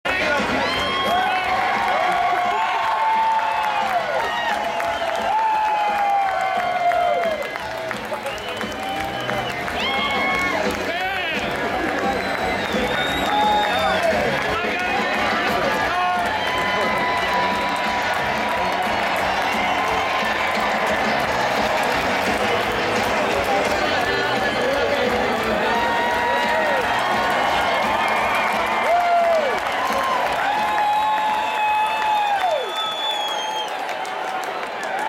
Bud Adams, Owner of the Titans, Celebrates 11/15/09 with a Flippant Celebration
Bud Adams celebrates at the end of the Titans vs. Bills game in Nashville, TN. This was right before he started sticking his middle finger up at the Buffalo fans. I just stopped recording then looked up and he was doing it. I turned my camera back on and was in such a hurry forgot to hit record. LOL!! Big mistake!! bird